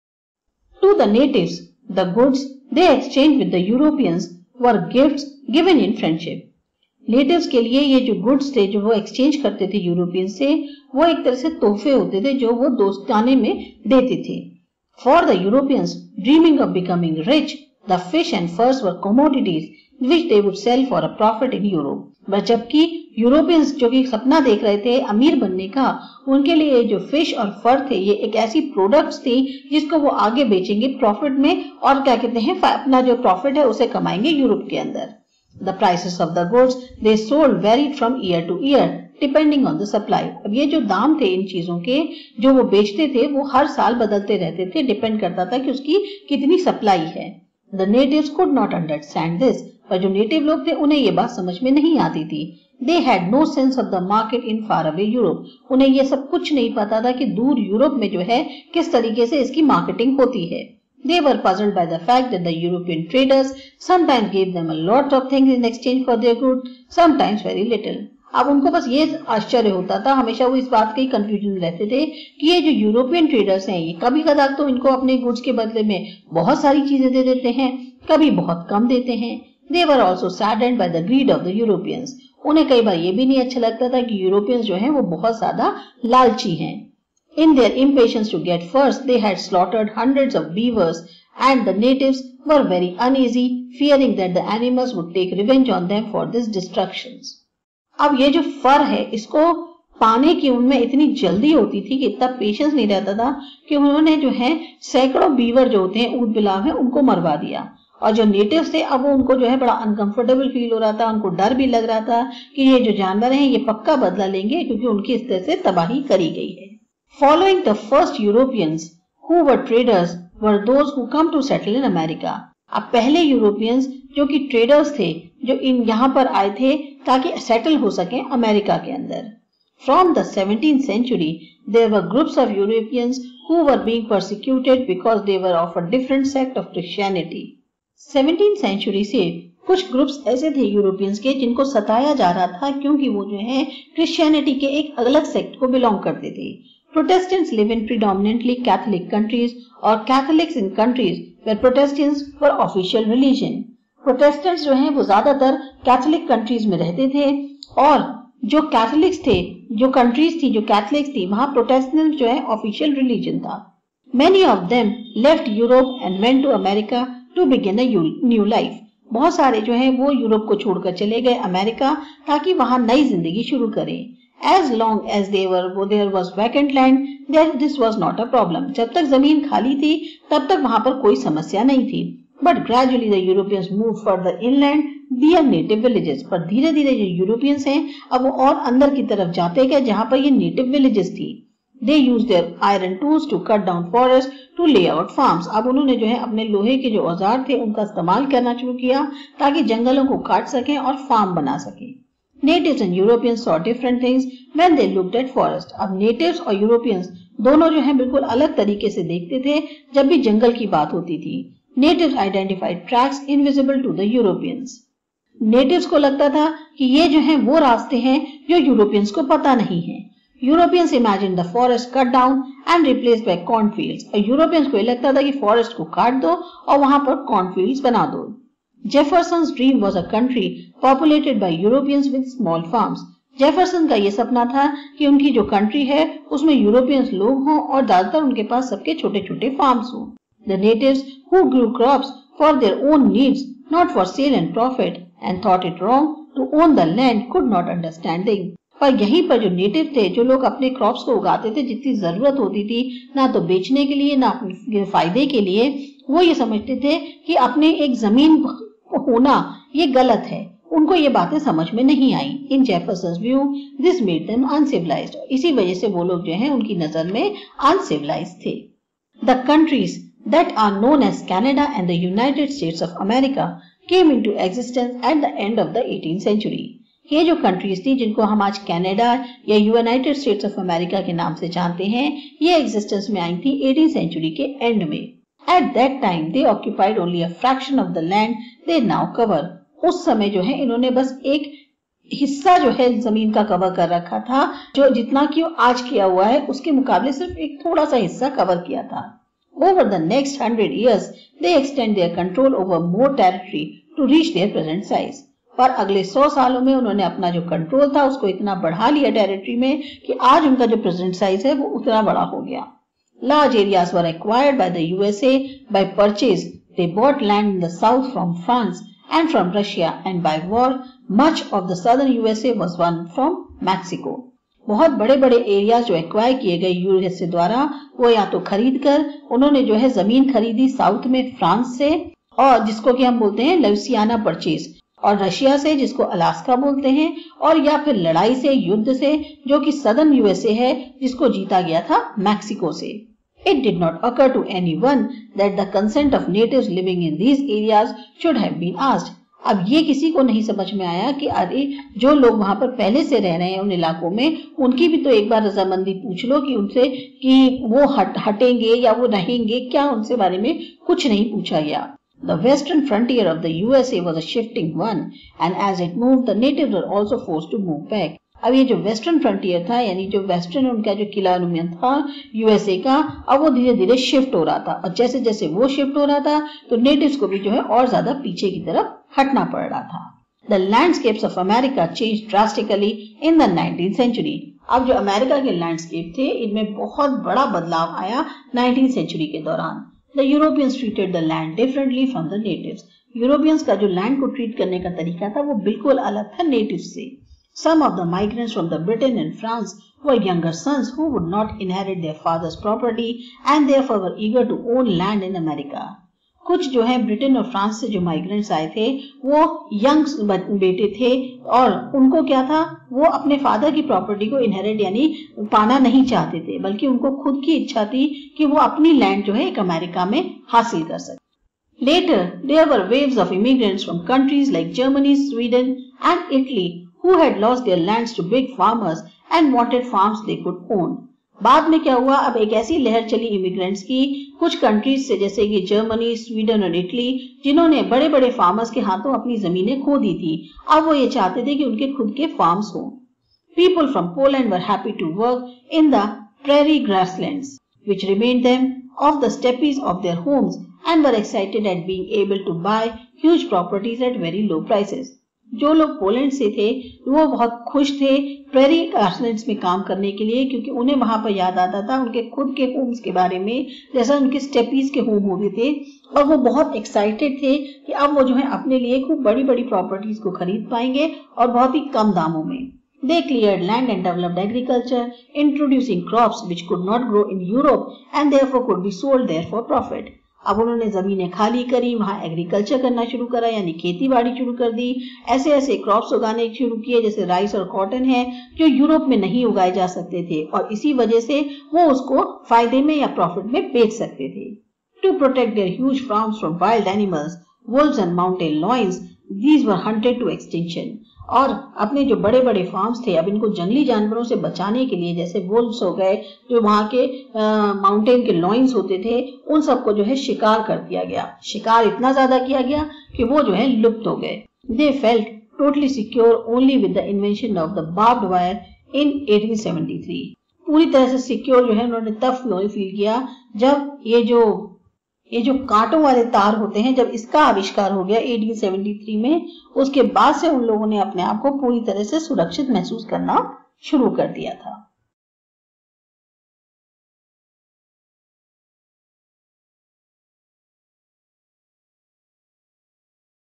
U4 Theme 10.4 Displacing Indigenous Peoples (History, CBSE, Grade 11) Easy explanation in Hindi
Subject - History.
Book - Themes In World History.
Unit 4 - Towards Modernisation.
Theme 10 - Displacing Indigenous Peoples.

PHDMP1012-A05